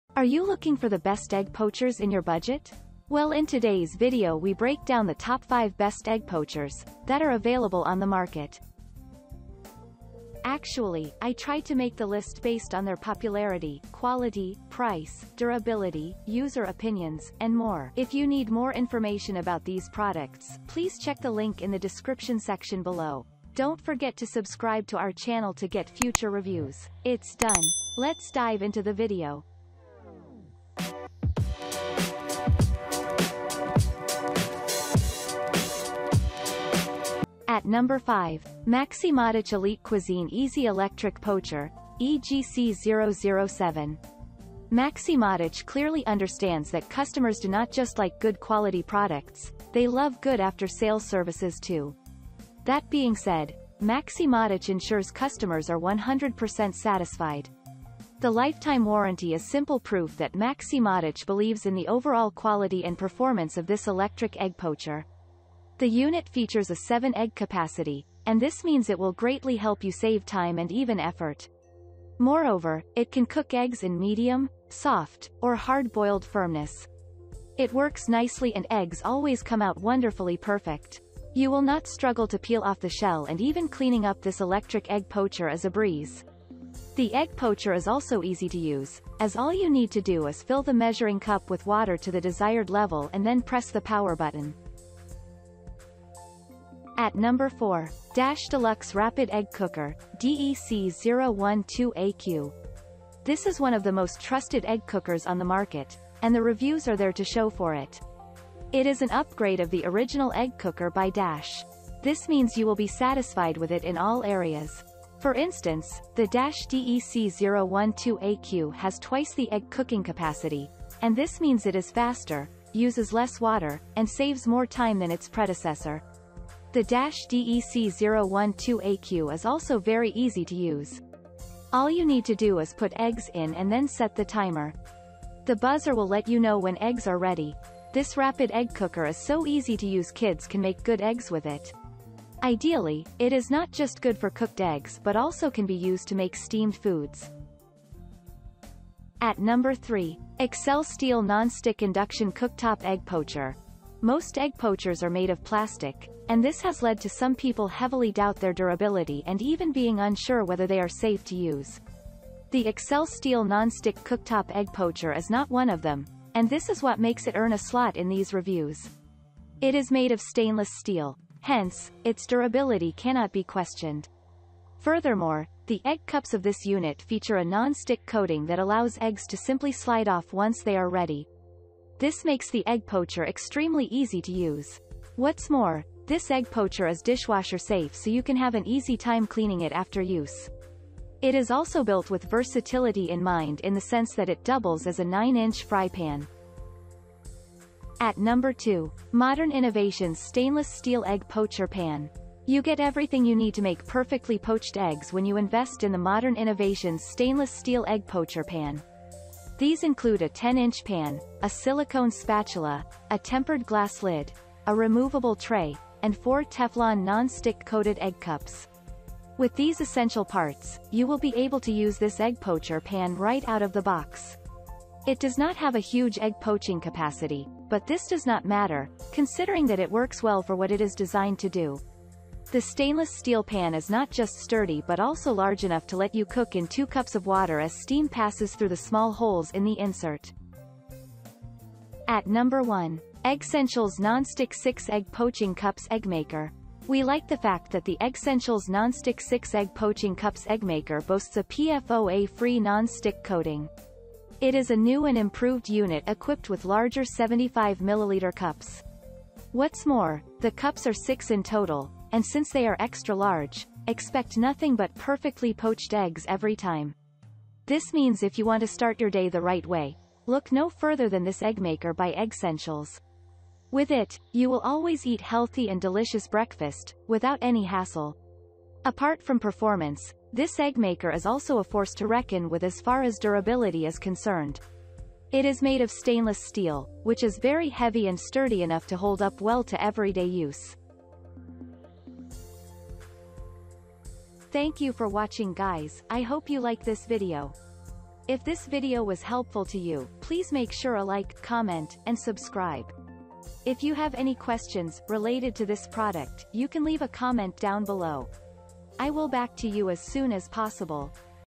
Top 5 Best Egg Poachers 2022 Reviews
▶️ 5. Maxi-Matic Elite Cuisine Easy Electric Poacher (EGC-007).

▶️ 4. Dash Deluxe Rapid Egg Cooker (DEC012AQ).

▶️ 3. ExcelSteel Non Stick Induction Cooktop Egg Poacher.

▶️ 2. Modern Innovations Stainless Steel Egg Poacher Pan.

▶️ 1. Eggsentials Nonstick 6 Egg Poaching Cups Egg Maker.